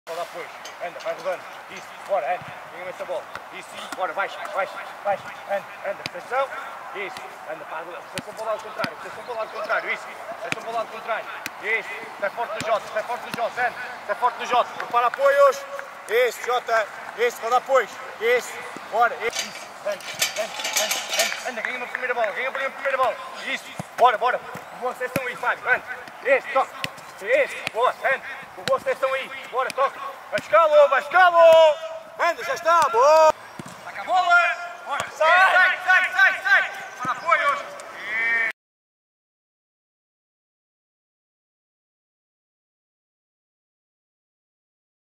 IFT | PERSONAL COACH - CENTRE BACK DEFENDER
Brief images of the central back defender training session, where small technical-tactical details were developed!

IMPROVE YOUR TALENT|
IMPROVE YOUR FUTURE|